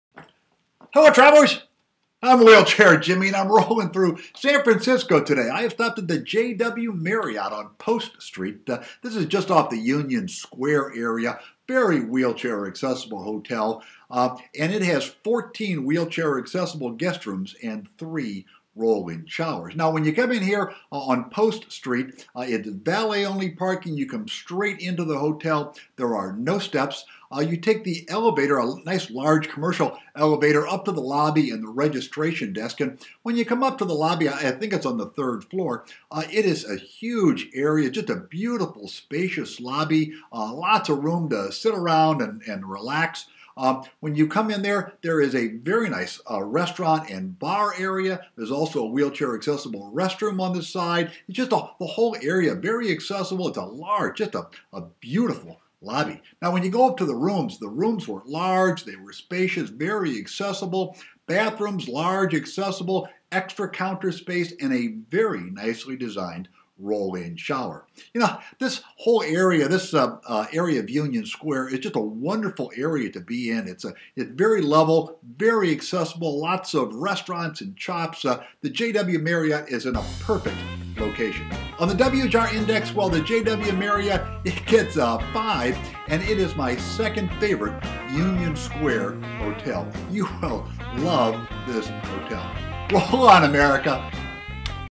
Wheelchair Accessible Hotel Reviews - JW Marriott Hotel on Post San Francisco, CA
Check out my video reviews of wheelchair accessible hotels, restaurants and attractions in the San Francisco area.